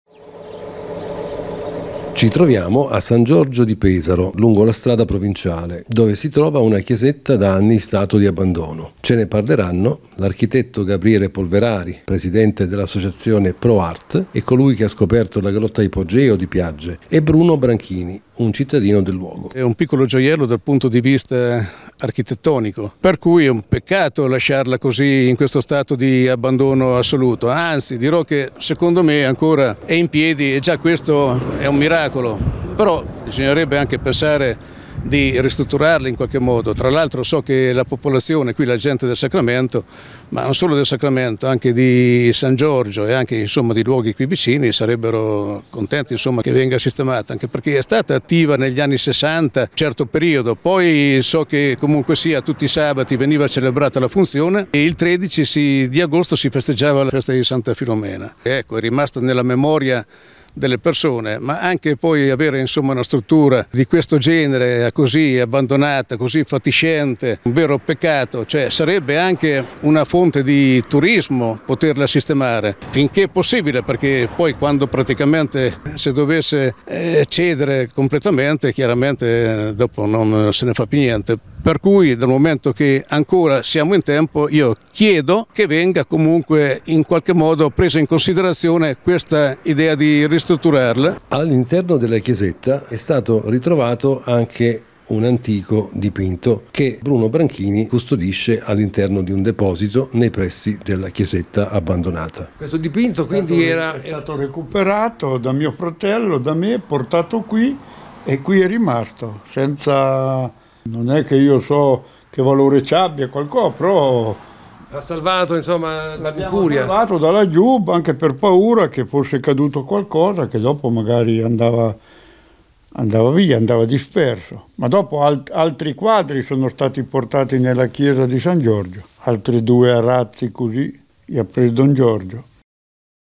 San Giorgio: "Restaurare chiesetta a rischio crollo"
Appello dei cittadini di San Giorgio di Pesaro che chiedono la restaurazione della chiesetta prima che sia troppo tardi.